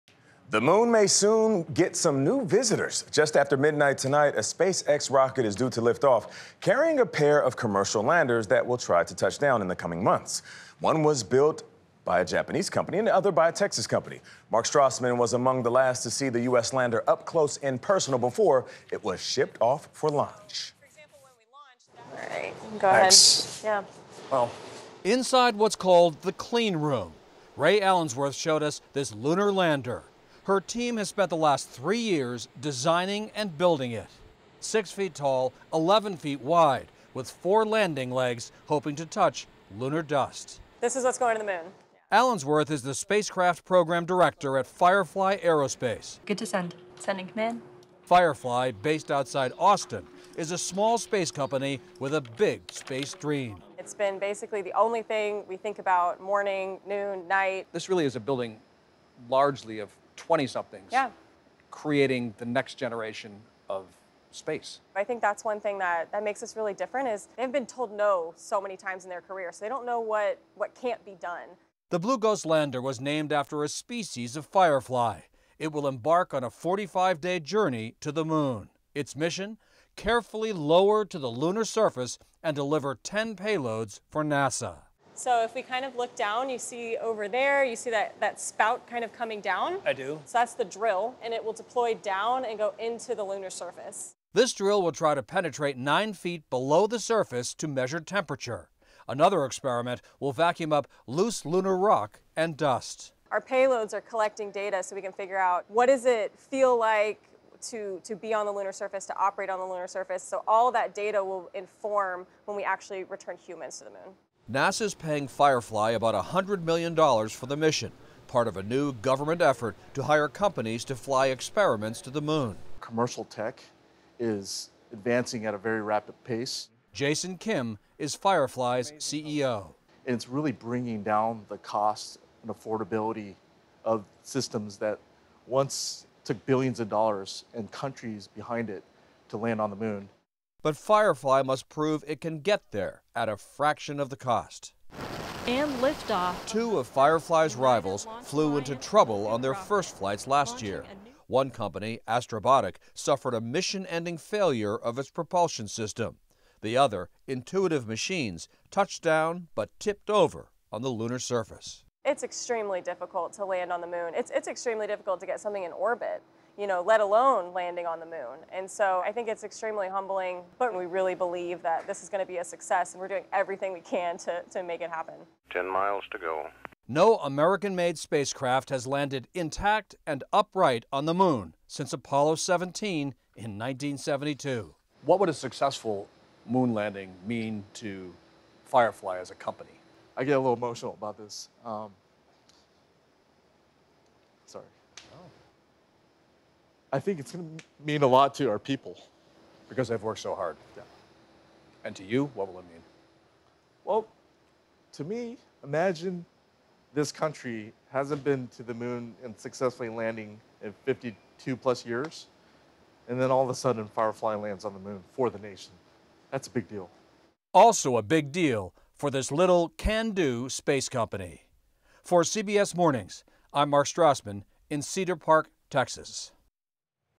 SpaceX to launch two lunar landers in new mission to the moon
Just after midnight Wednesday, a SpaceX rocket is scheduled to lift off, carrying a pair of lunar landers that will try to touch down on the moon in the coming months. One was built by a Japanese company and the other was built by a Texas company.  Their mission is to carefully lower to the lunar surface and deliver 10 payloads for NASA.